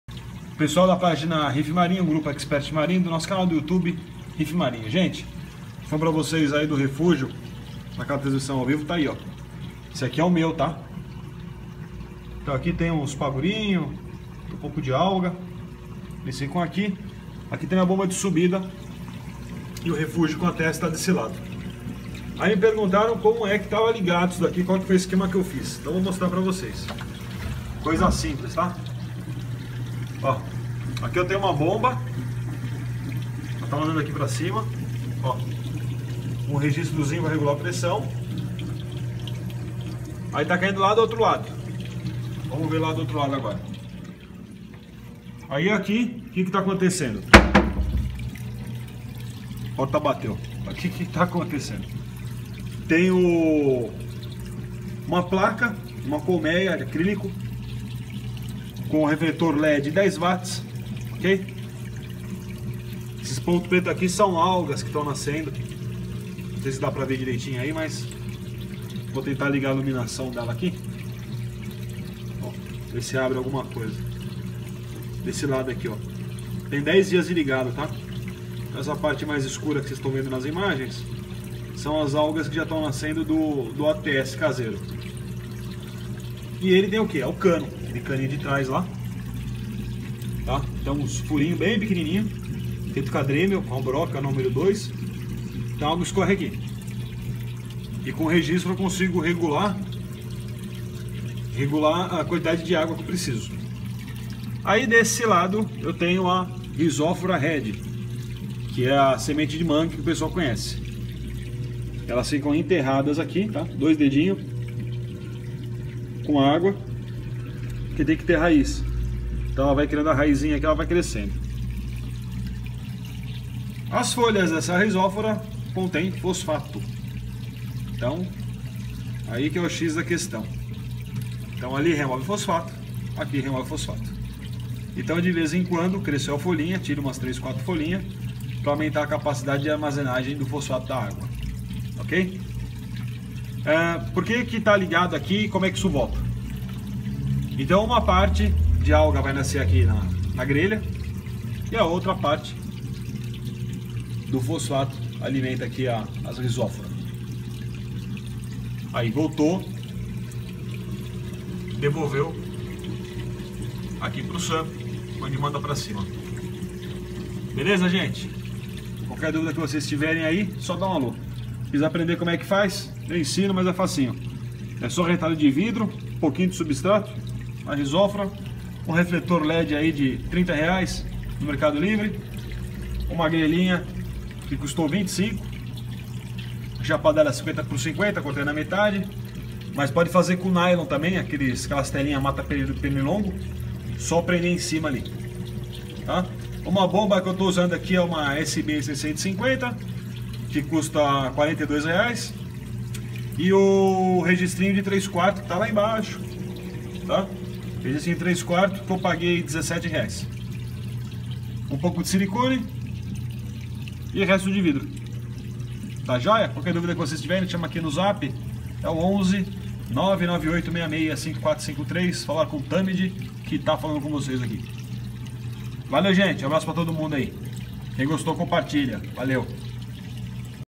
TUDO como fazer um Filtro de Algas - ATS PARA AQUARIO MARINHO 50,00 - DY
Fácil de fazer é muito barato !!

ATS PARA AQUARIO MARINHO 50,00 - DY

Livro sobre Aquarismo Marinho direto da Amazon ! EM PROMOÇÃO ! Lançamento Mundial !

Curso para Crescimento no youtube !